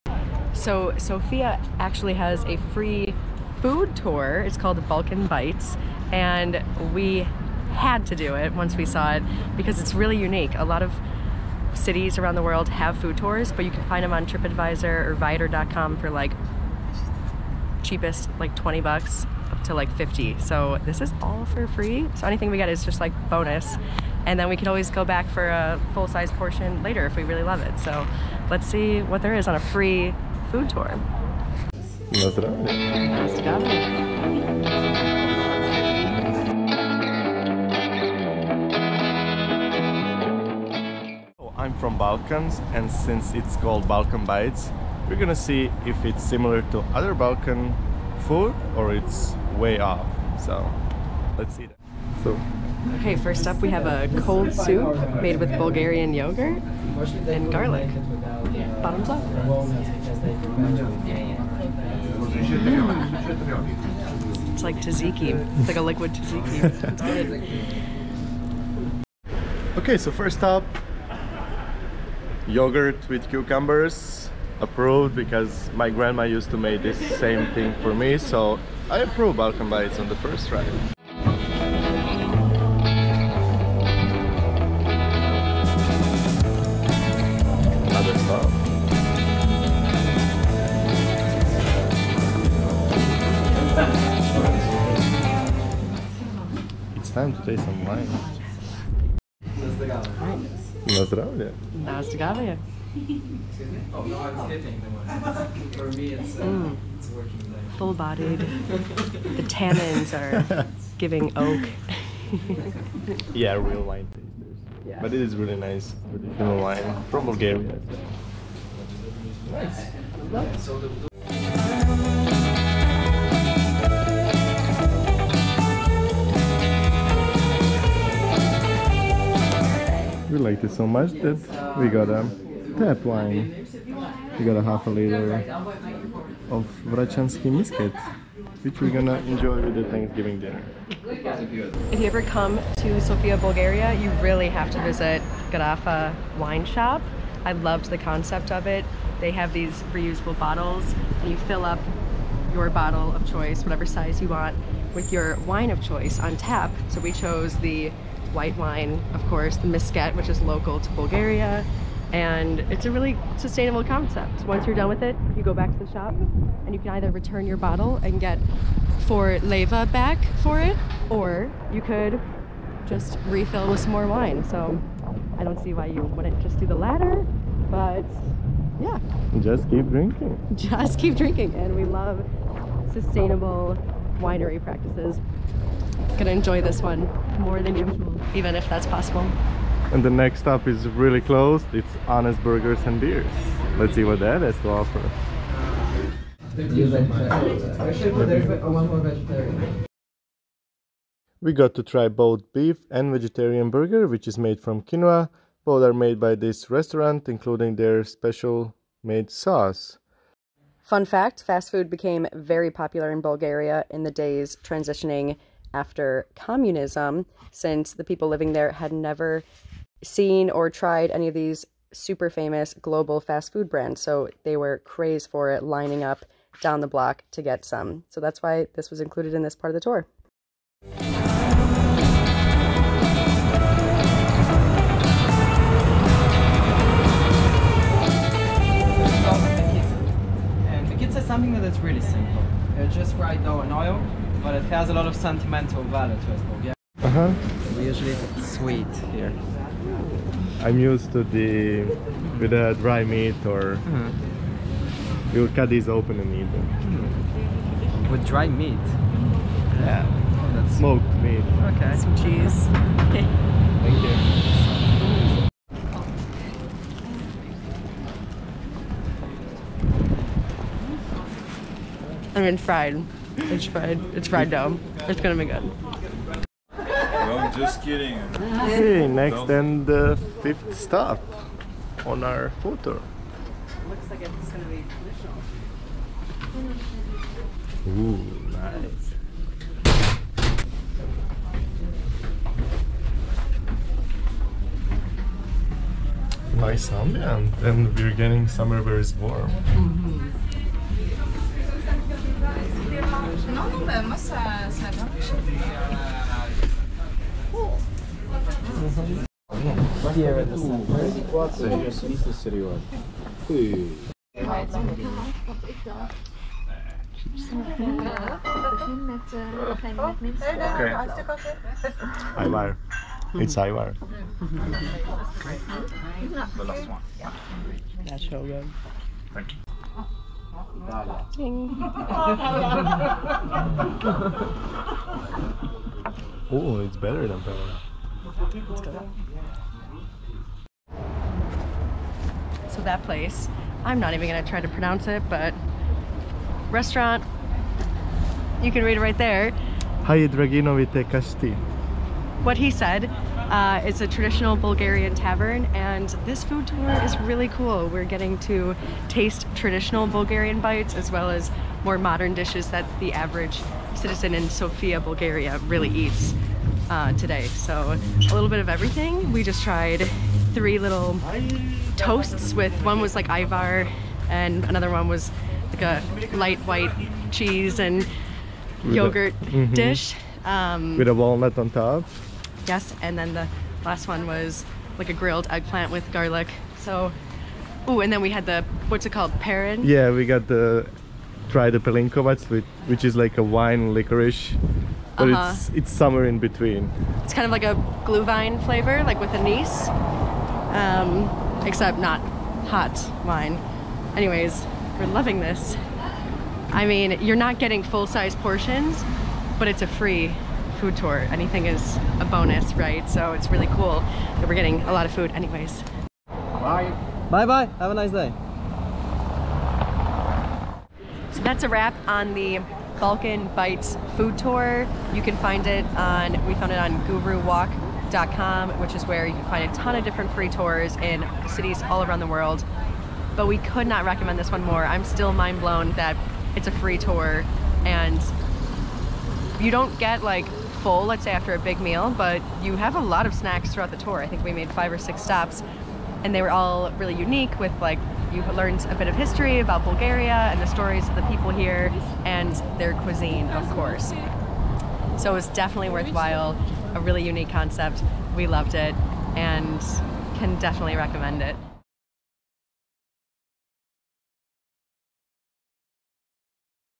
FREE Bulgarian Traditional Food 2022 | Food Tour with BALKAN BITES
Hello guys today we bring you with us on trying Traditional Bulgarian food COMPLETLY FREE, wondering how? Today we show you how, with the company in Sofia named BALKAN BITES you can acces free Food Tour where they bring you to try some of the most popular Bulgarian Traditional dishes like Their famous Bulgaria Joghurt Soup, you get to taste some nice Bulgarian wine local Bulgarian Street Burgers. We bring you throught everything in this Free Sofia Food Tour.